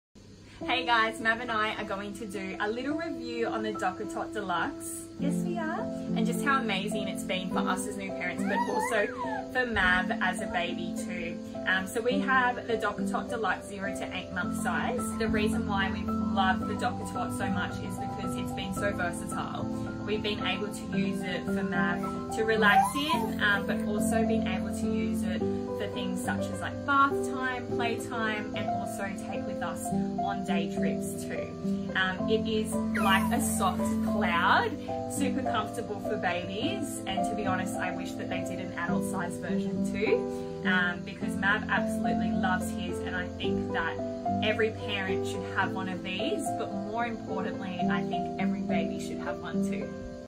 New Mum DockATot Review
@felicityegginton shares why she loves her DockATot⁠